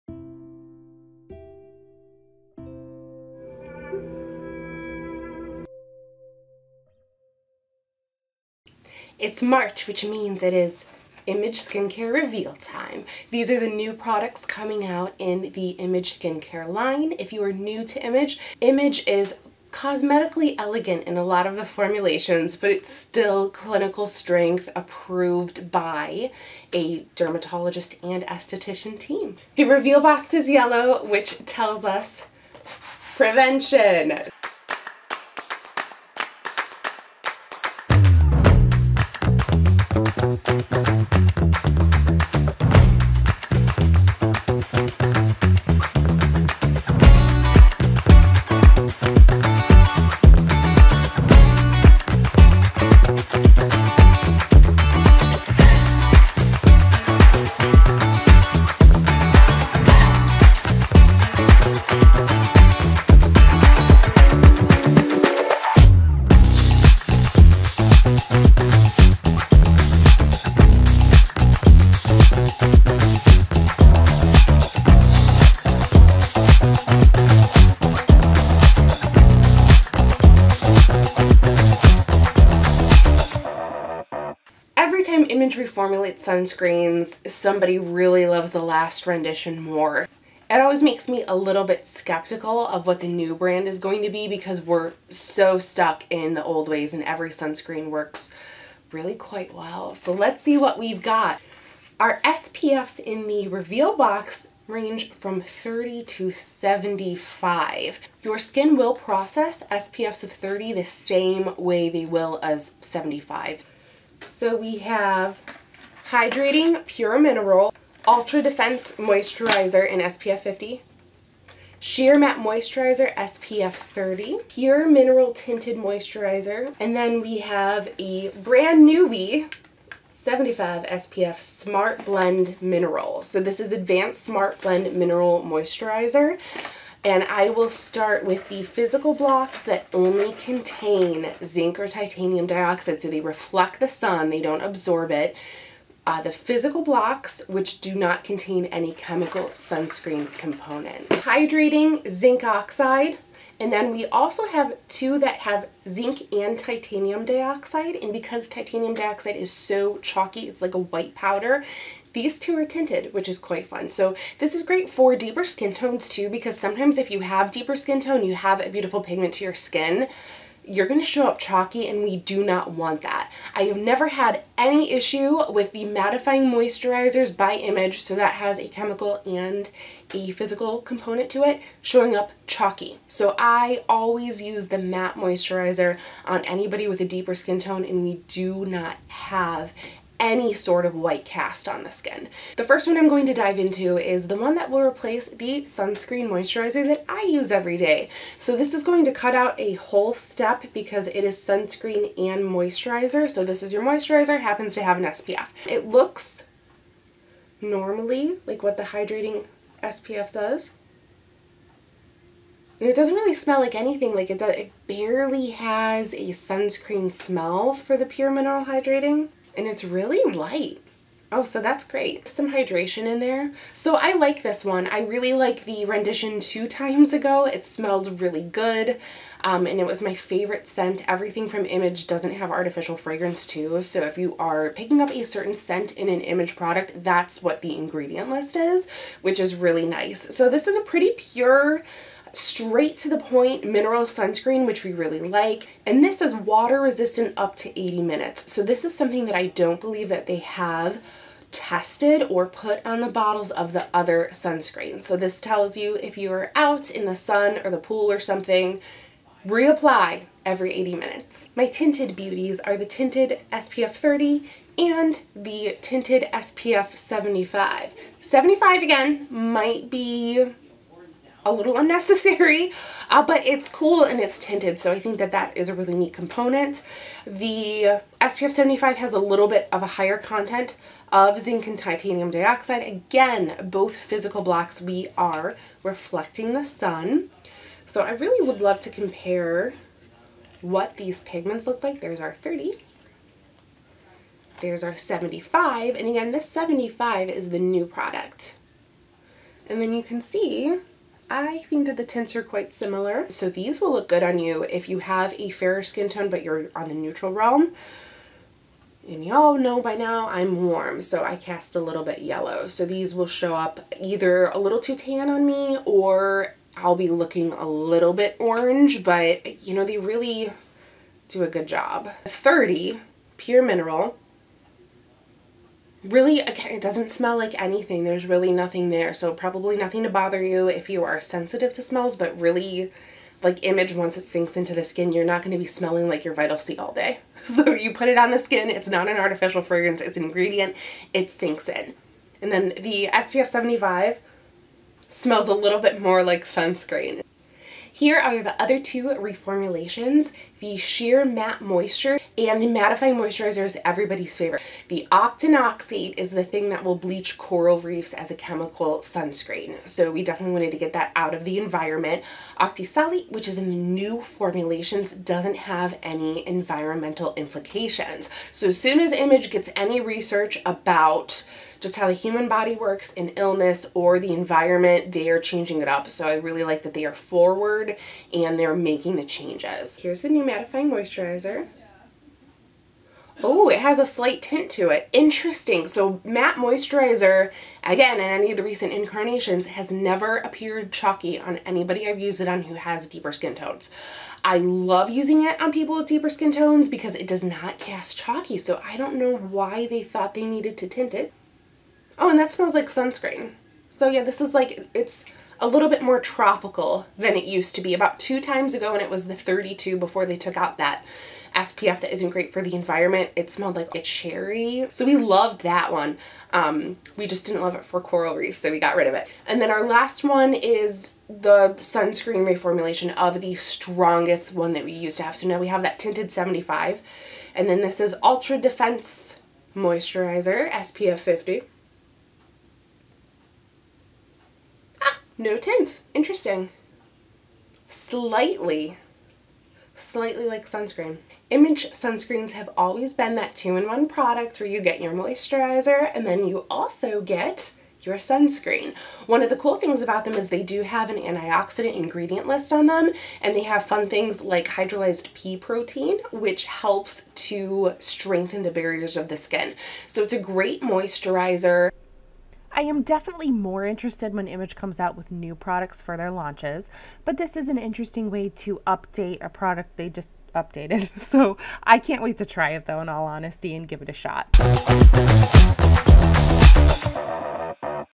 Image Skincare *New Products* Launch 2024
Join me as I reveal Image's new skincare! They have reformulated the sunscreen moisturizers *again*` with cooler twists than usual (hint: tint!)